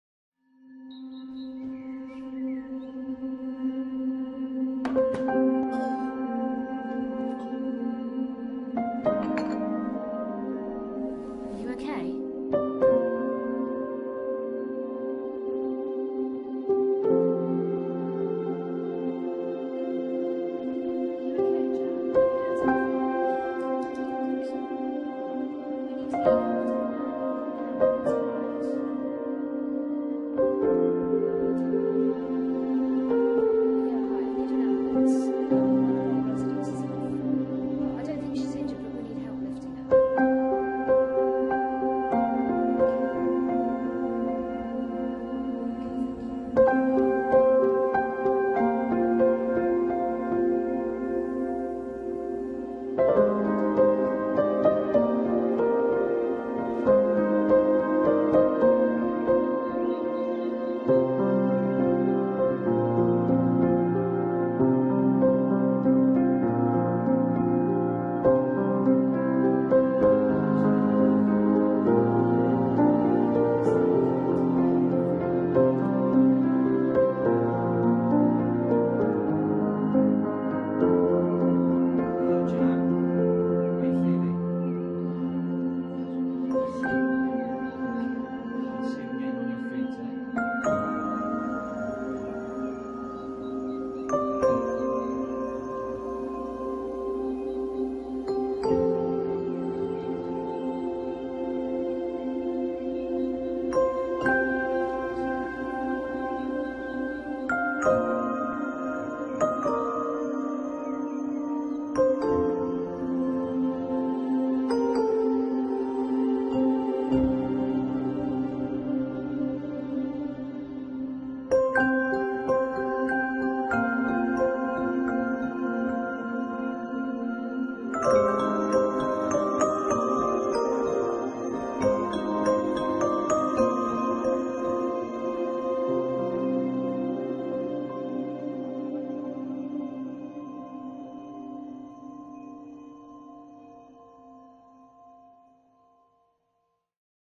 mangar camel emergency lifting cushion
Mangar Camel is a multi-compartment inflatable lifting cushion designed to gradually inflate using a hand operated controller. It safely lifts a person who has fallen to the ground into a sitting position. Manual handling injuries normally associated with a carer lifting are now no longer.